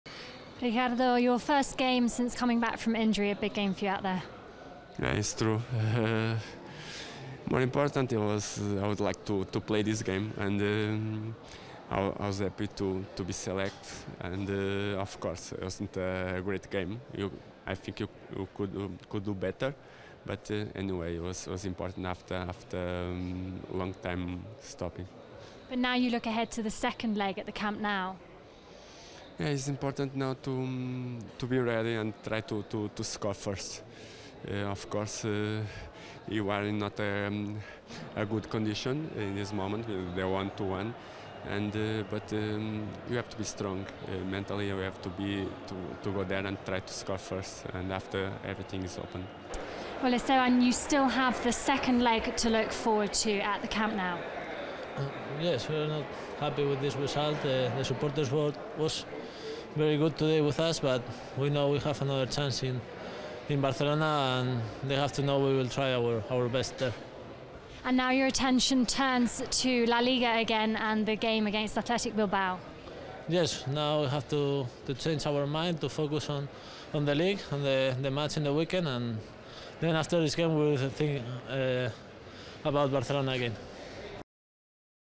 Ricardo Carvalho and Granero addressed the media in the mixed zone after the match.
Carvalho:"We can always do better. We were fine and playing a tight and balanced game, but Puyol's goal after half-time changed everything. Barcelona became more confident and their game improved. We'll do everything we can to turn the tie around."